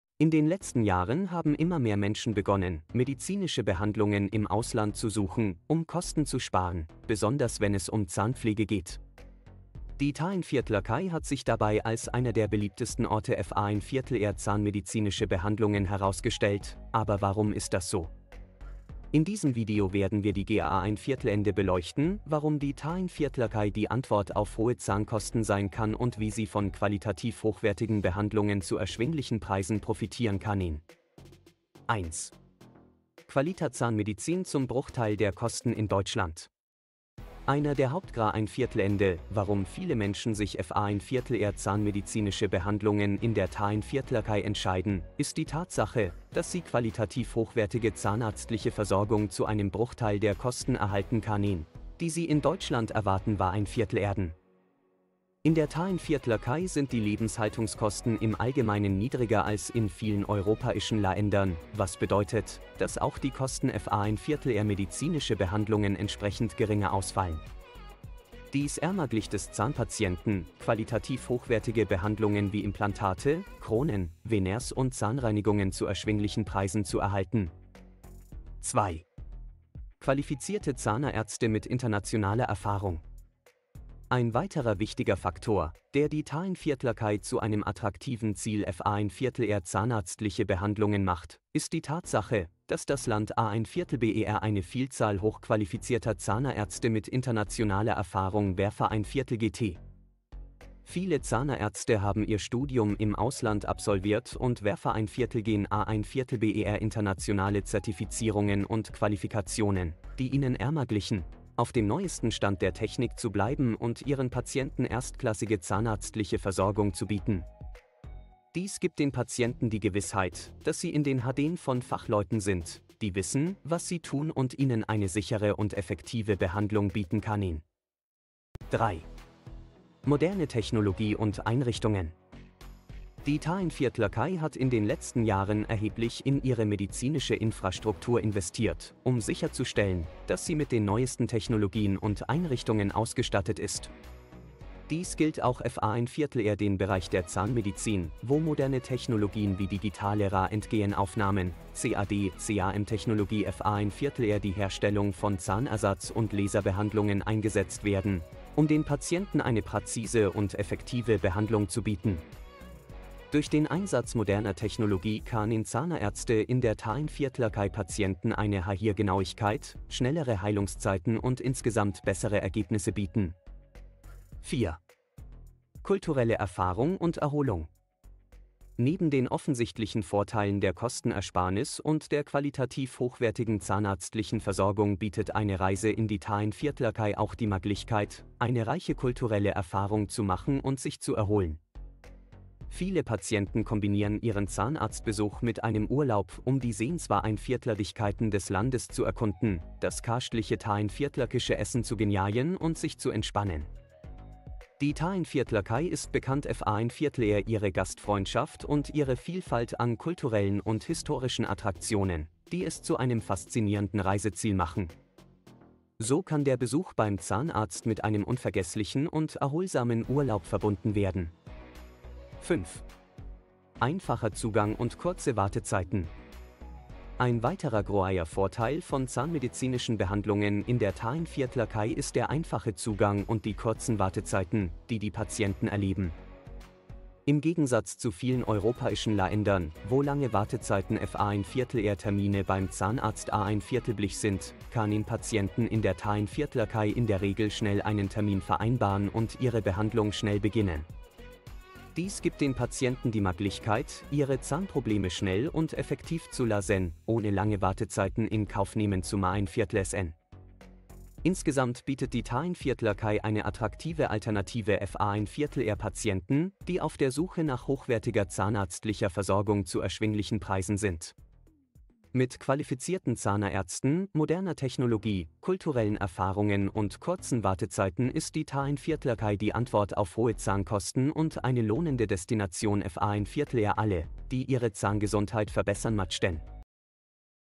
Zahnmedizin in der Türkei - Lohnt sich die Reise für günstige Behandlungen?
In den letzten Jahren haben immer mehr Menschen begonnen, medizinische Behandlungen im Ausland zu suchen, um Kosten zu sparen - besonders wenn es um Zahnpflege geht. Die Türkei hat sich dabei als einer der beliebtesten Orte für zahnmedizinische Behandlungen herausgestellt, aber warum ist das so? In diesem Video werden wir die Gründe beleuchten, warum die Türkei die Antwort auf hohe Zahnkosten sein kann und wie Sie von qualitativ hochwertigen Behandlungen zu erschwinglichen Preisen profitieren können. Türkei, Zahnpflege, medizinische Behandlungen, Kostenersparnis, Ausland